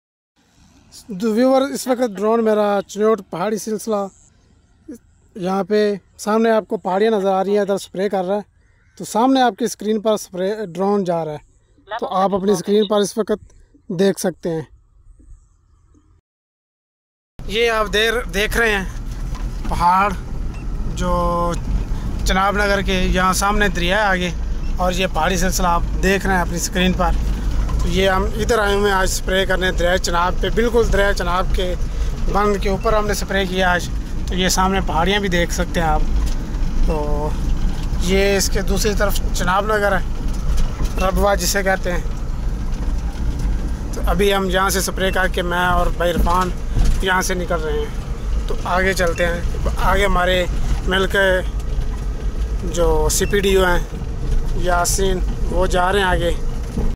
Chinot Hills Pr Drone Spray | Chenab Riverr Pr Drone Ky Sath Spray Kea
Chiniot Hills Drriye Chenab Riverr,
Drone Spray,
Rizwan Punjabi  Drone Wala
Drone Spray By Rizwan